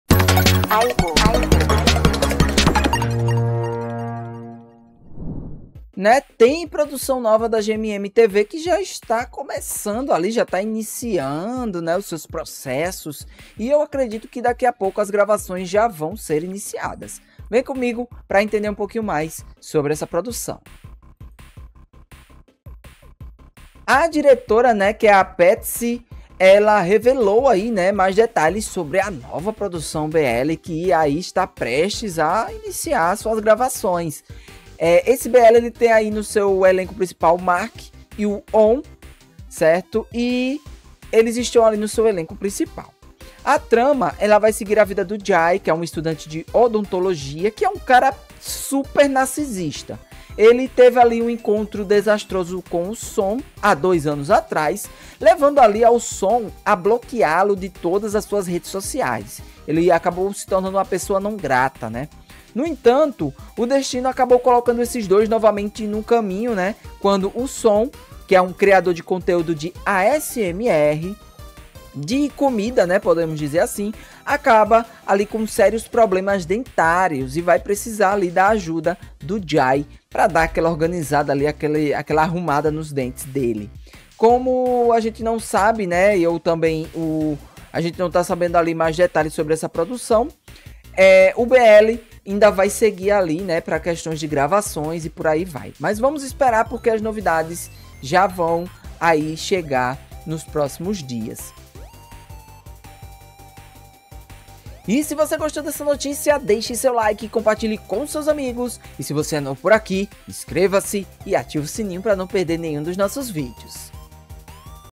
🚨Pepzi Banchorn se prepara para iniciar as gravações da série BL "Sweet Tooth Good Dentist"
🔴 Estamos ao vivo de segunda a sexta, às 19h, com tudo que acontece no universo BL!